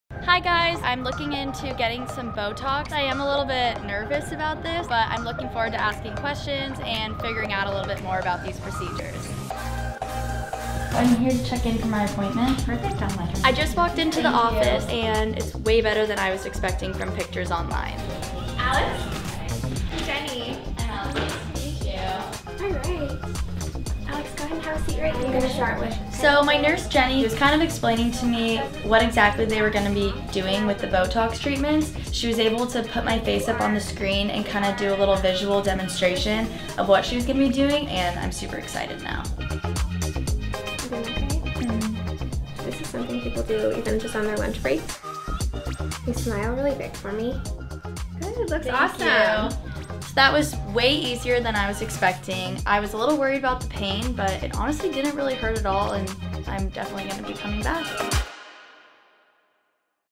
Botox Treatments with Parson Skin Center
See a patient testimonial with what you can expect for Botox treatment at Parson Skin Center.